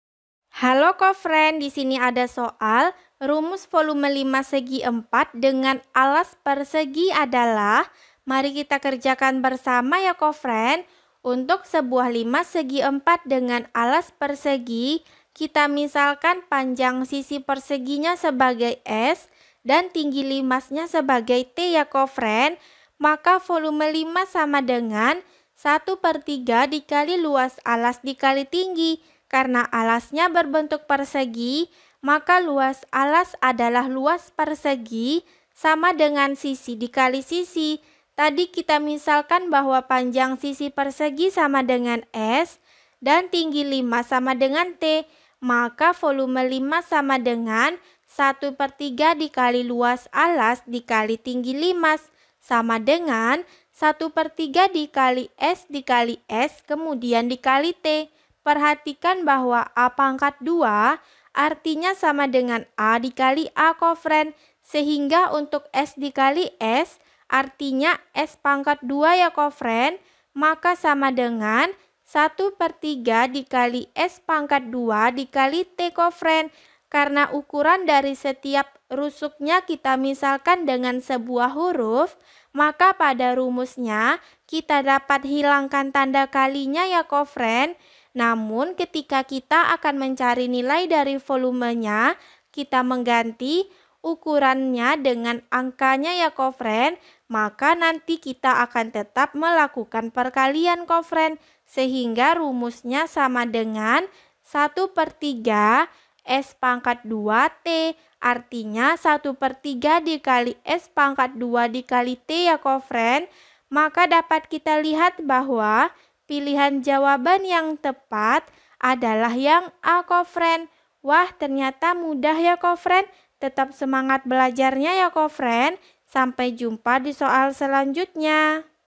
Rumus volume limas segiempat dengan alas persegi adalah....
Topik atau Materi: Volume dan Luas Permukaan Limas Segiempat - Bangun Ruang - Geometri - Matematika Kelas 5 SD -

Sumber Soal: Matematika SD Ruang Guru Kelas 6 , N/A

00:00 : Bedah Soal
01:00 : Konsep, rumus dan pengertian Volume dan Luas Permukaan Limas Segiempat
01:18 : Langkah penyelesaian soal
01:08 : Jawaban akhir
01:38 : Penutup